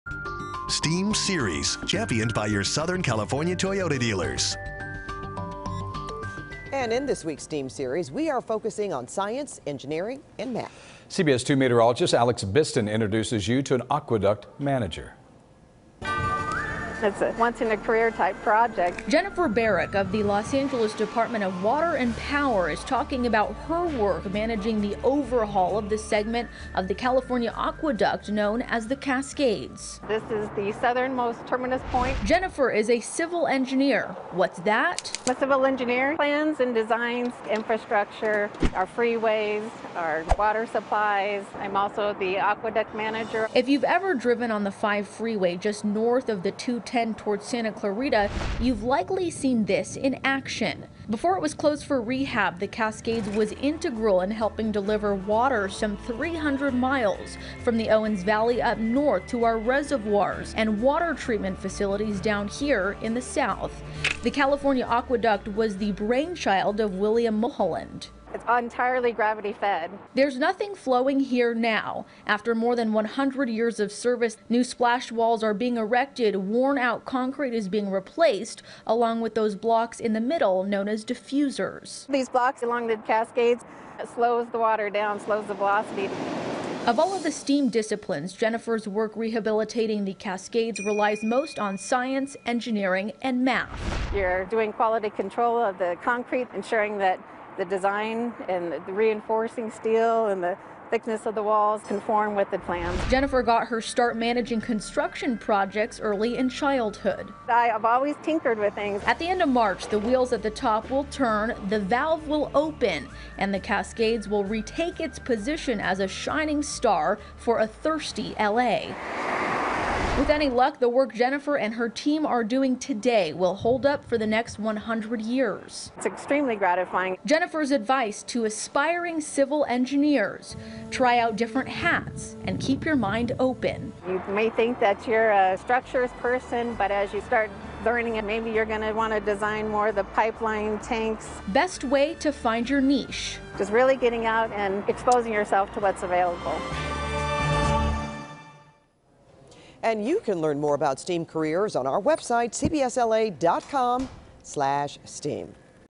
STEAM: Meet An Aqueduct Manager
Jennifer Barrick is a civil engineer for the Los Angeles Department of Water and Power managing the overhaul of the segment of the California Aqueduct known as The Cascades. Barrick said her work as an aqueduct manager relies on her knowledge of science, engineering and math.